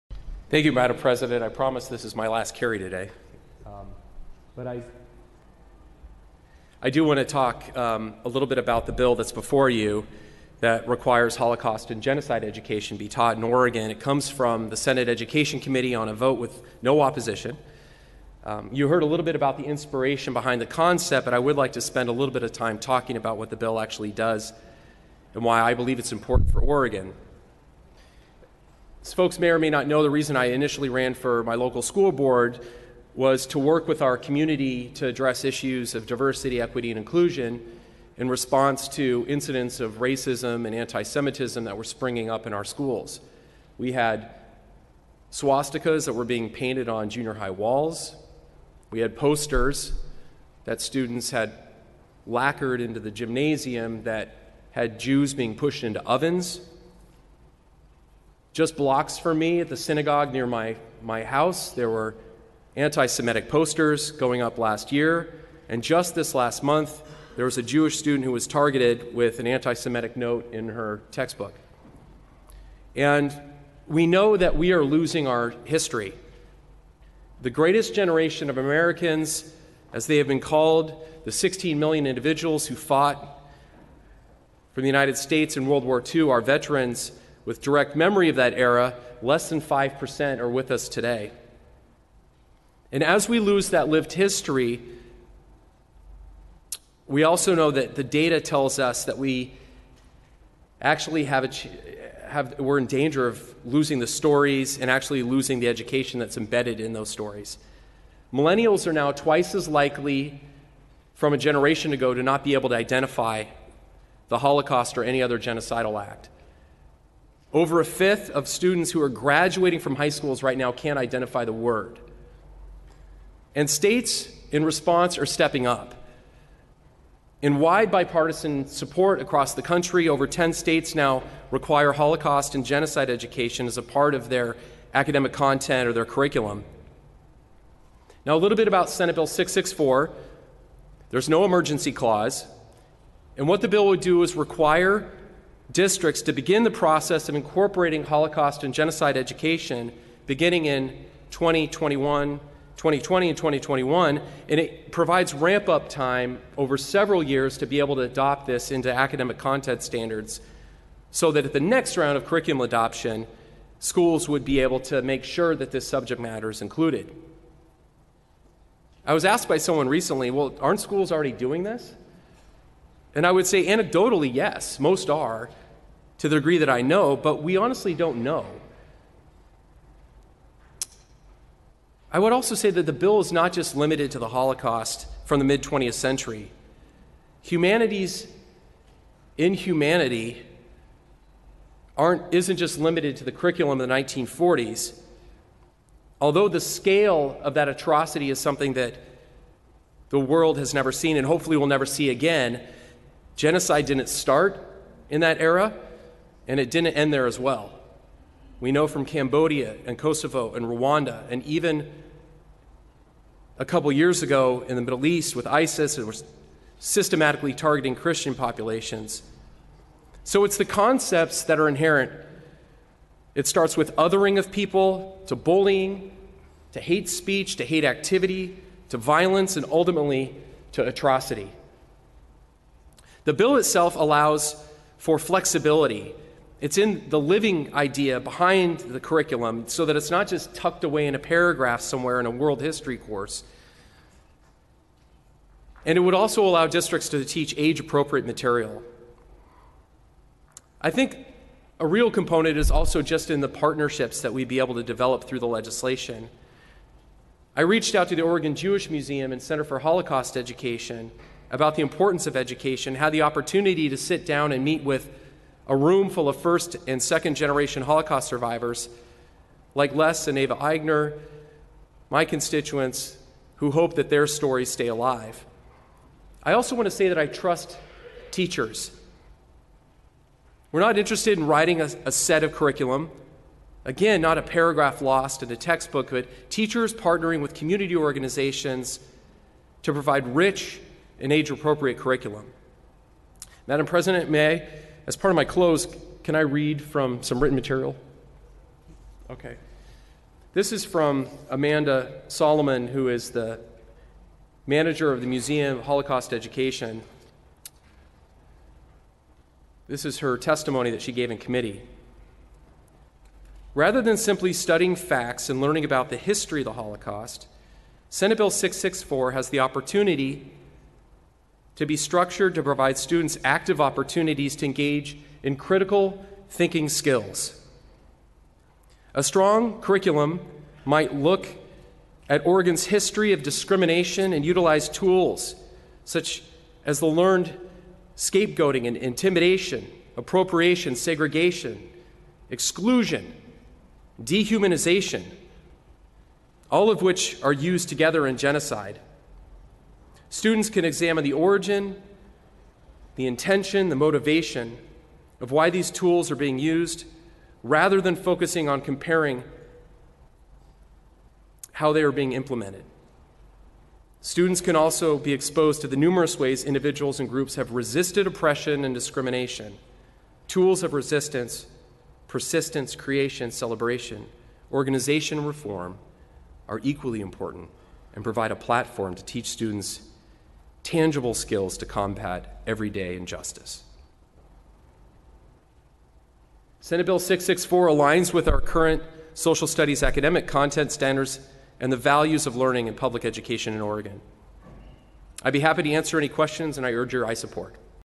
Senator Wagner Floor Speech on SB 664
Senator Wagner presented Senate Bill 664 to the Oregon Senate on March 12, 2019. Senate Bill 664, passed during the 2019 Legislative Session, requires all Oregon school districts to provide Holocaust and genocide education.